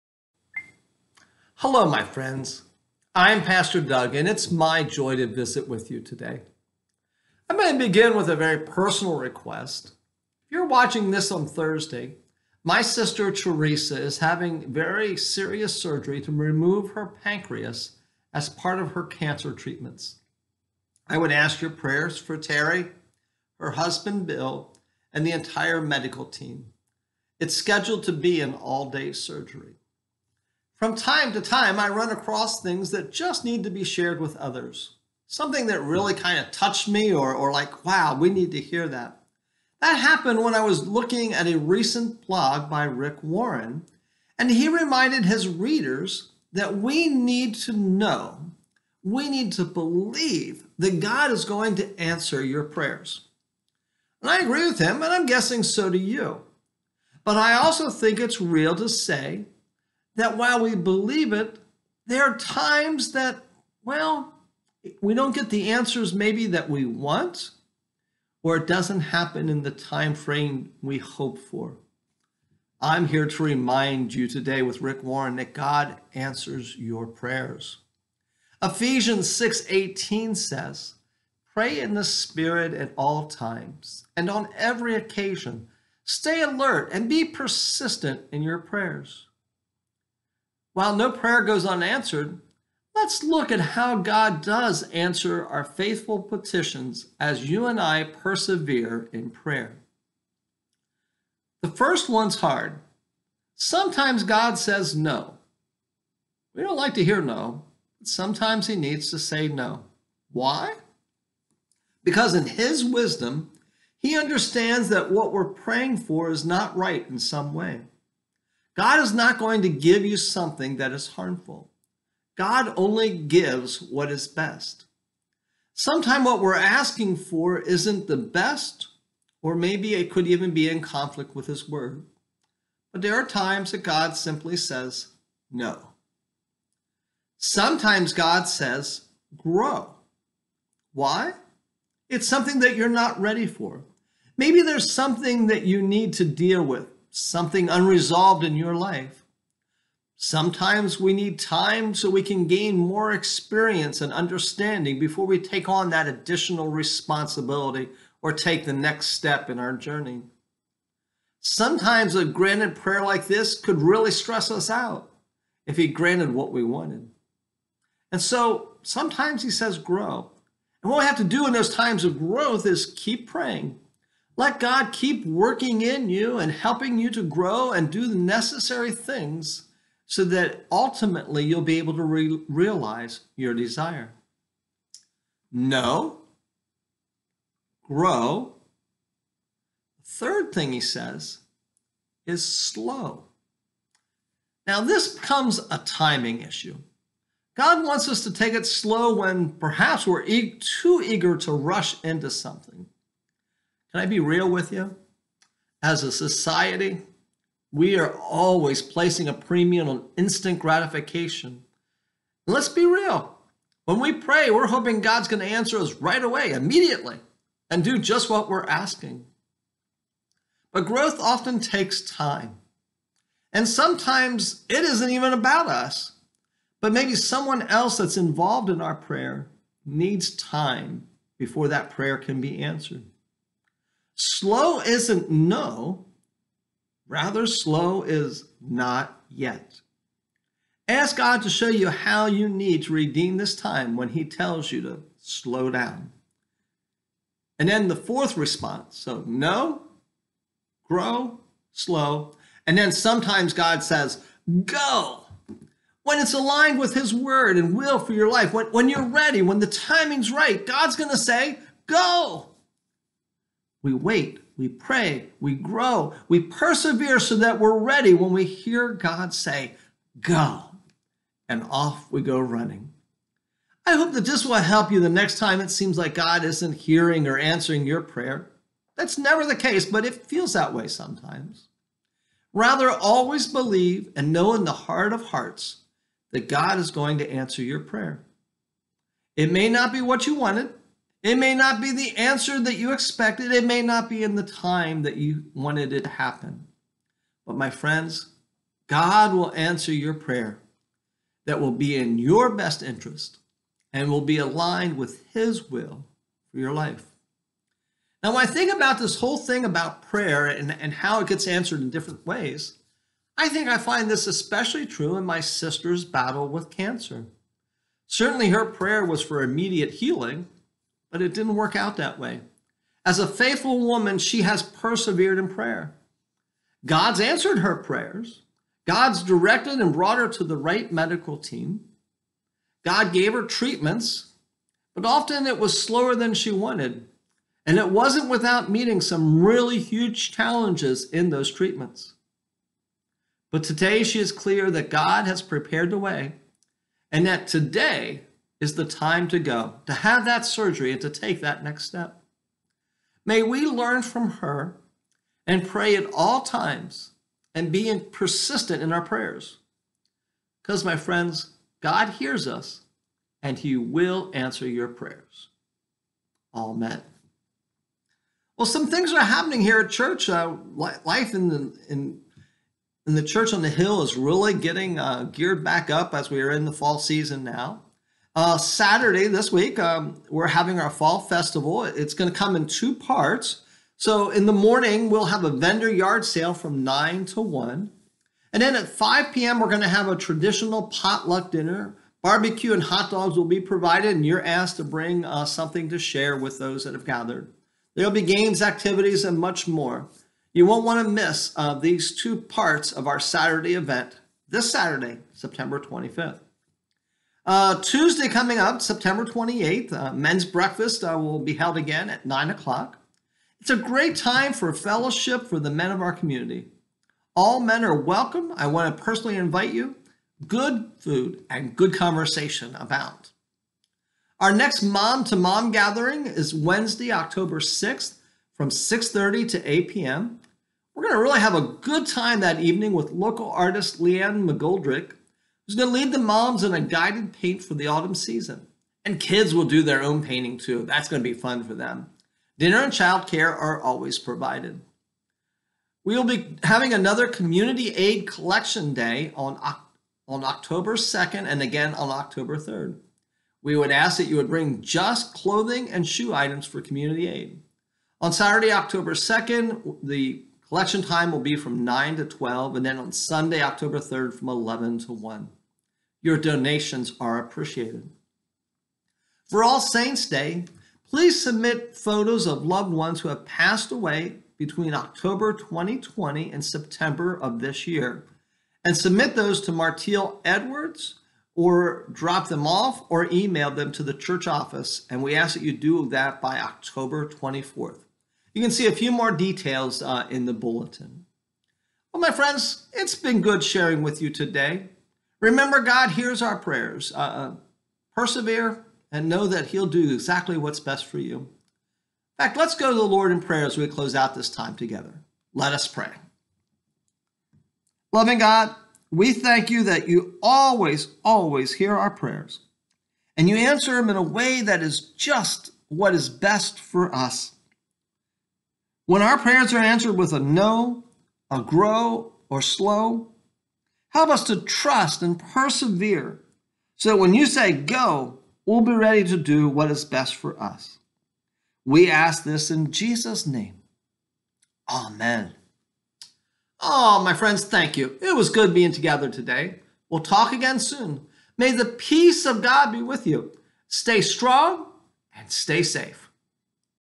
Weekly Message
A Word from Pastor Doug - this week Pastor Doug reminds us that our prayers are always heard and answered--just not always the way we want or expected.  Listen as he shares 4 ways God answers our prayers.